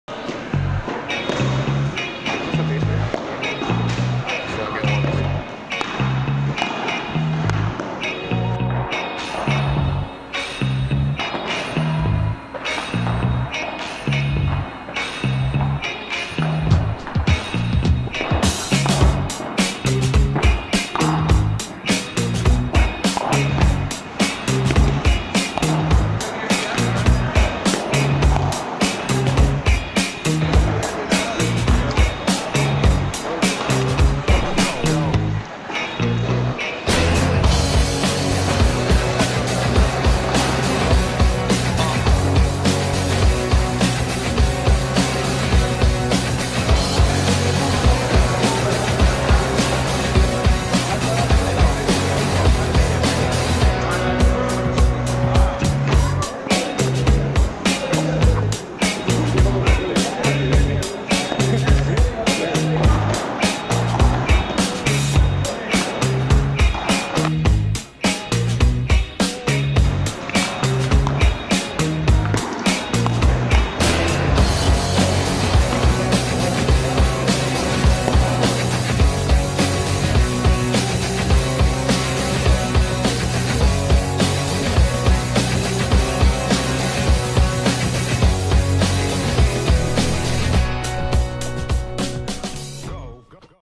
Bernipádel, Moringa Time
Torneo Bernipadel en Sanset Padel Indor. Invitando a aprovechar la ventana metabólica con Moringa.